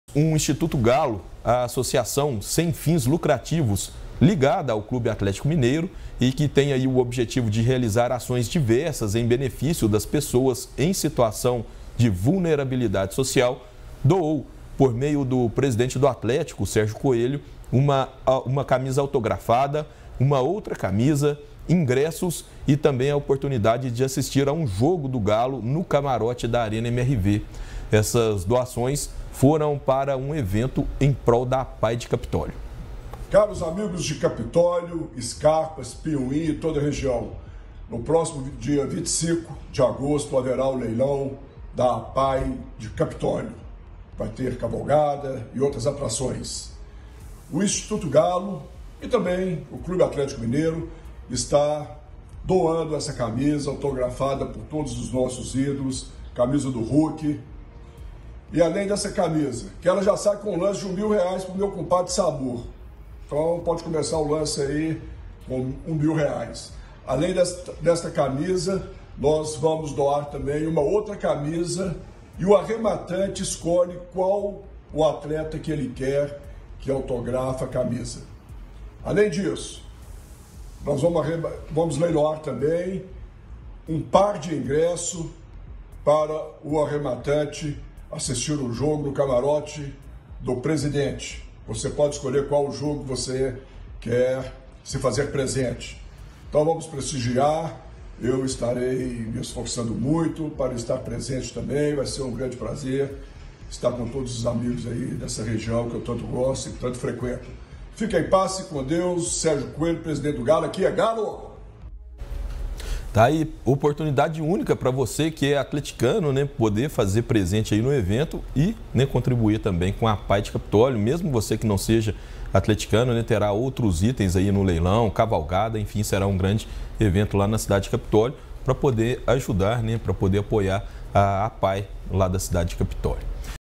INSTITUTO GALO DOA CAMISA AUTOGRAFADA PARA SER LEILOADA EM PROL DA APAE DE CAPITÓLIO
| REGIÃO |

O Instituto Galo, associação sem fins lucrativos ligada ao Clube Atlético Mineiro e que tem o objetivo de realizar ações diversas em benefício das pessoas em situação de vulnerabilidade social, doou, por meio do presidente do Atlético Sérgio Coelho, uma camisa autografada, outra camisa, ingressos e a oportunidade de assistir a um jogo do galo no camarote da Arena MRV. Essas doações foram para um evento em prol da APAE de Capitólio. Veja a fala do presidente do clube: